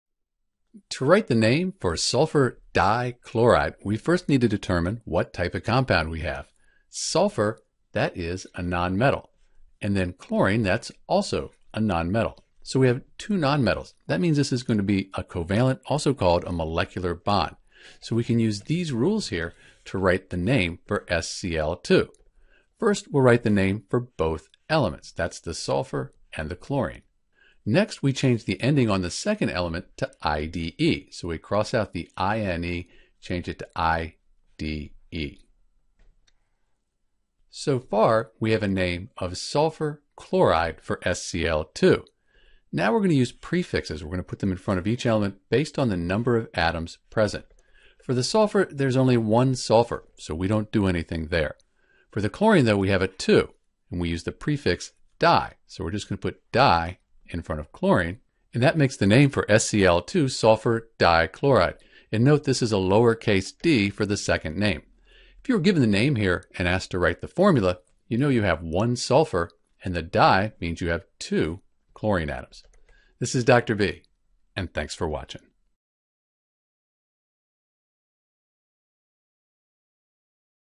How to Write the Name for SCl2
In this video we'll write the correct name for SCl2. To write the name for SCl2 we’ll use the Periodic Table and follow some simple rules.

Because SCl2 consists of two non-metals we don't need to balance the charges between atoms. Instead, we'll use prefixes to indicate the number of each element.

Mono = 1
Di = 2
Tri = 3
Tetra = 4
Penta = 5
Hexa = 6
Hepta = 7
Octa = 8
Non = 9
Dec = 10

--- Keys for Naming Molecular Compounds:---

1. Write the name for both elements.

2. Change the ending of the second element to ide.

3. Place prefixes in front of each element based on the number of atoms present.

4. The prefix 'mono' is only used on the second non-metal in the chemical formula.

5. There shouldn't be two vowels in a row. For example, when you have 'mono' in front of 'oxide' it is written 'monoxide', not 'monooxide'.

For a complete tutorial on naming and formula writing for compounds, like Sulfur dichloride and more, visit:

Drawing/writing done in InkScape Screen capture done with Camtasia Studio 4.0. Done on a Dell Dimension laptop computer with a Wacom digital tablet (Bamboo).ical name for SCl2